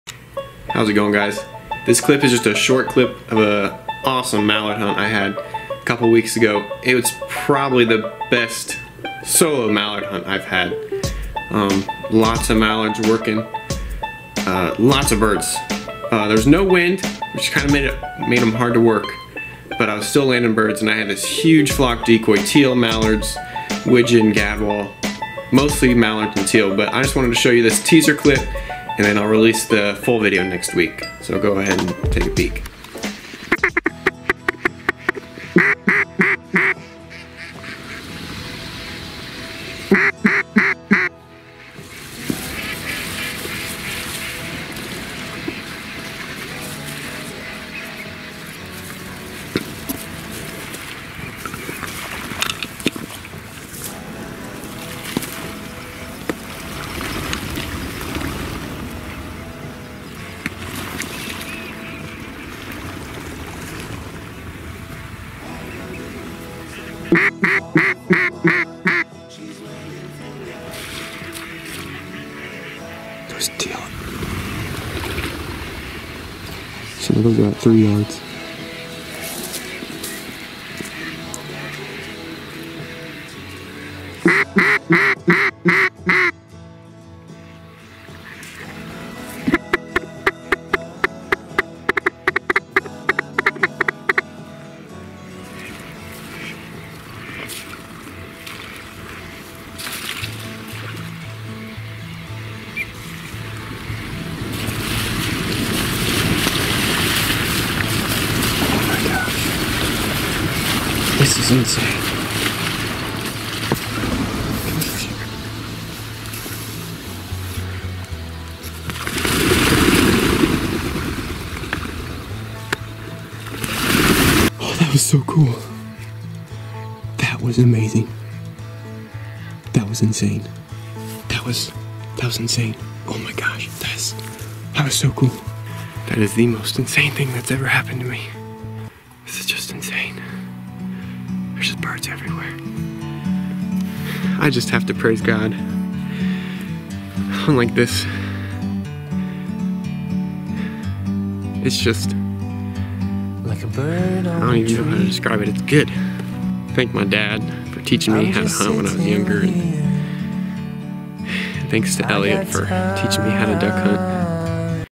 200 Ducks Land in an ICE HOLE, Teaser
This video is a great clip of waves of ducks landing on an ice hole I was hunting. It is after shooting time and I am just enjoying the incredible site. This video is a precursor to the hunt video which will be the next hunt video after this.